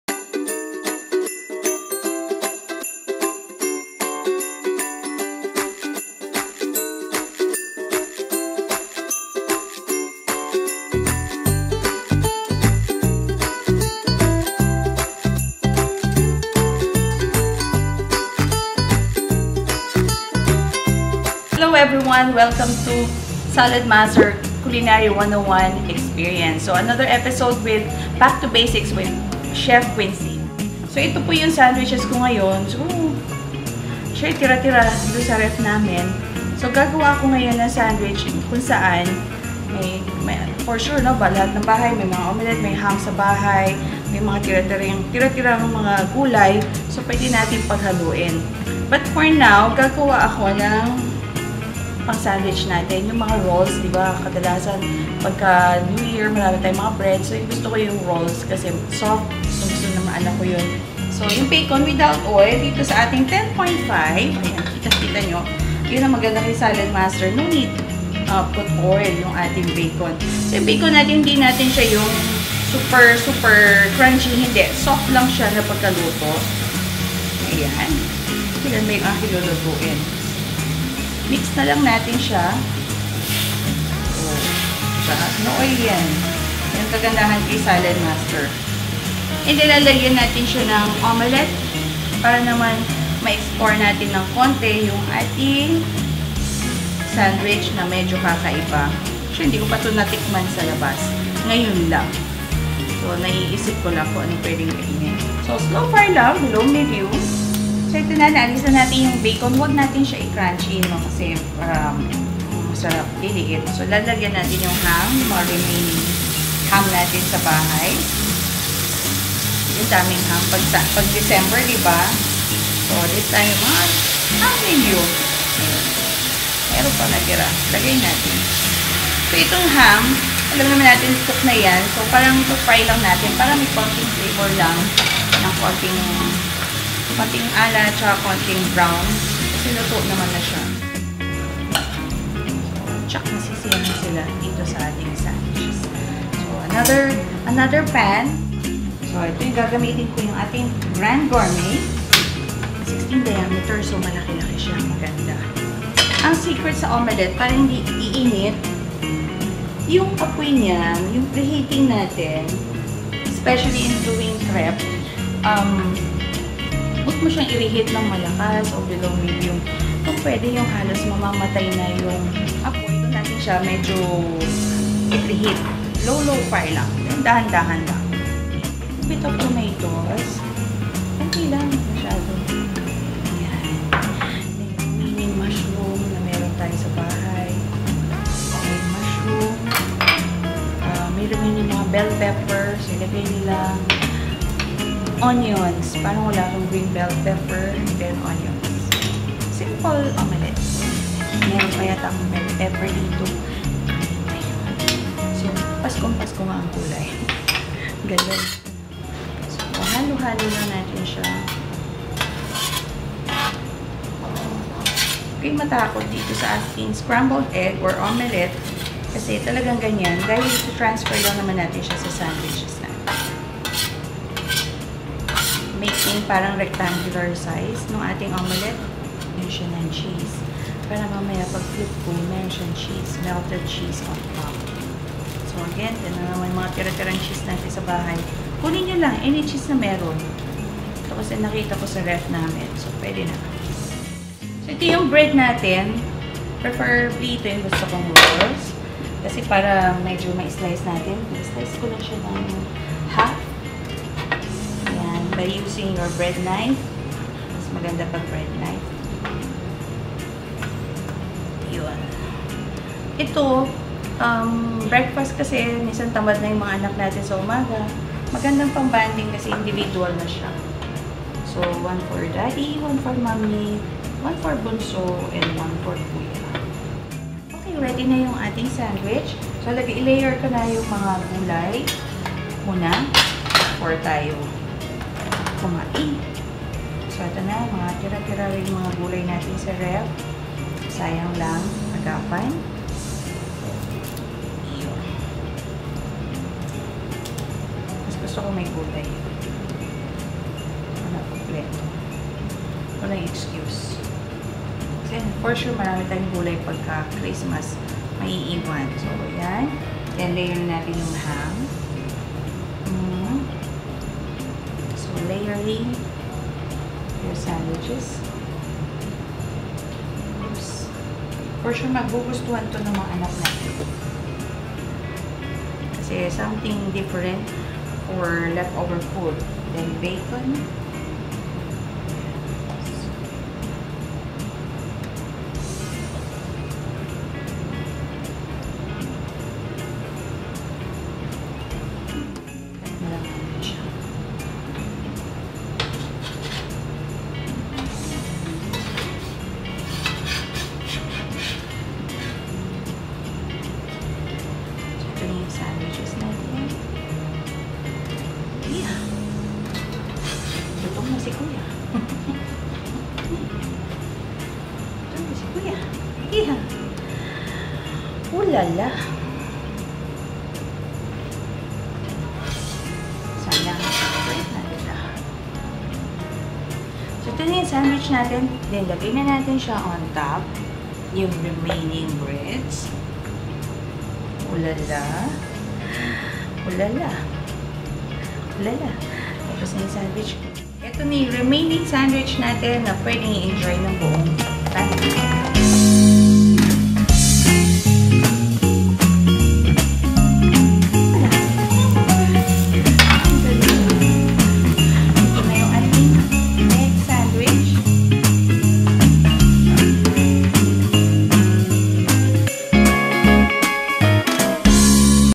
(Back to Basics with Queen C) EP 9. Ham and Cheese Omelet Bread Rolls
Flaky, soft, and buttery, these fresh dinner rolls are a perfect sandwich recipe that will win everyone's heart.